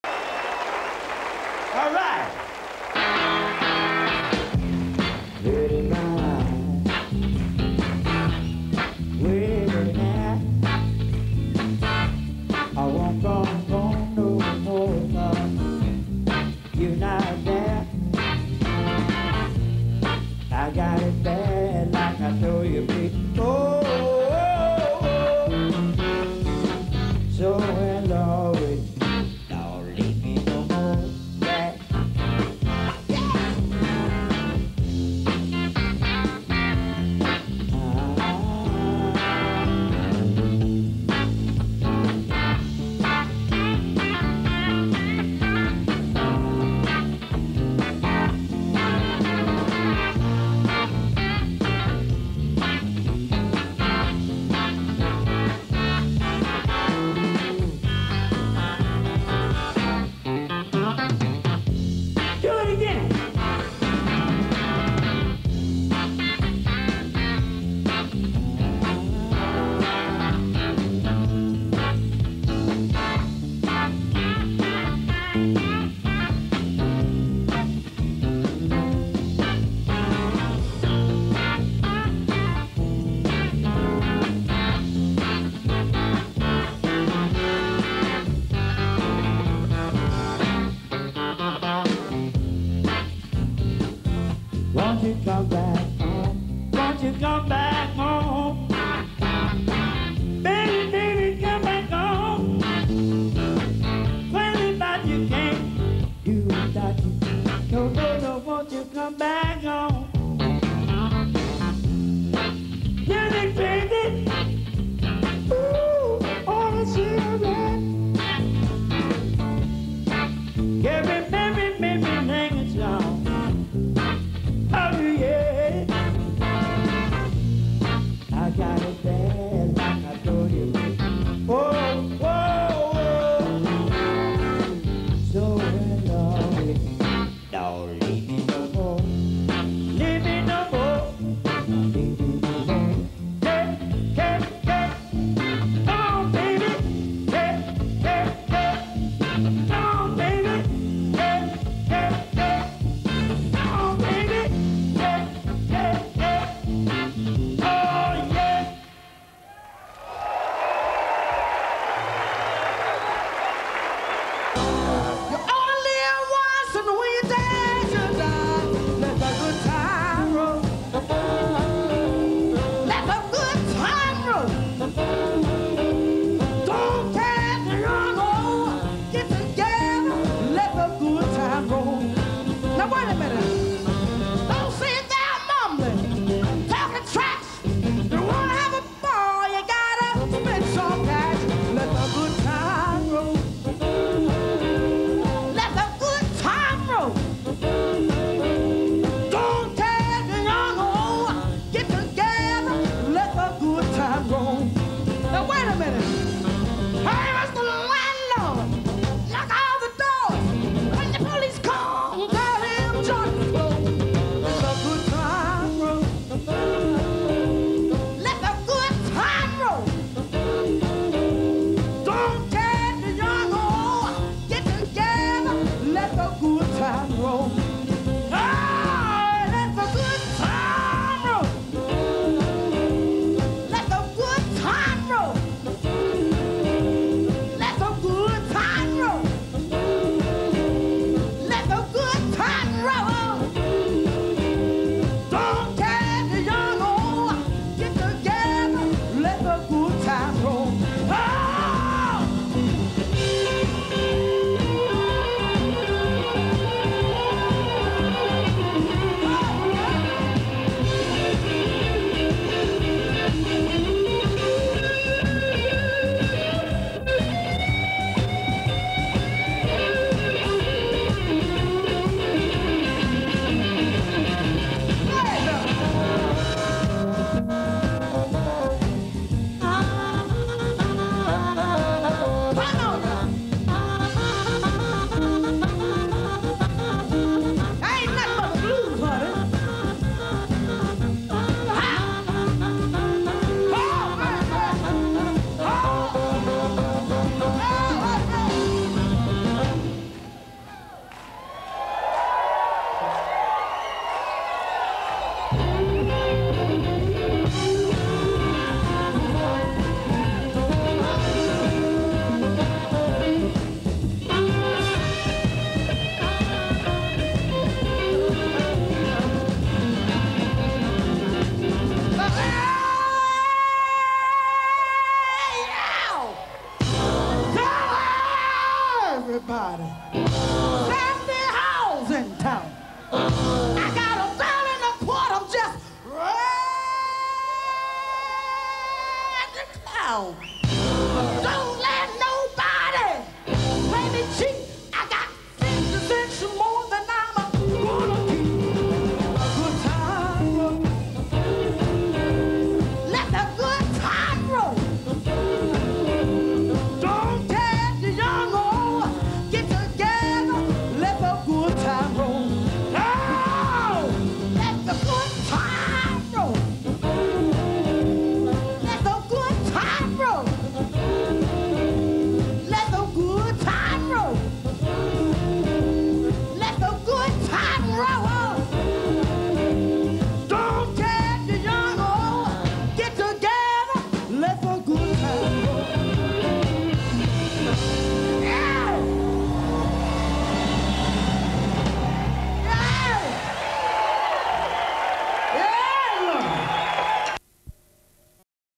Blues At Montreux.
Performances at Montreux  1990 from Snooks Eaglin ,  House Rocker Johnson & Blind Willie's Blues Review.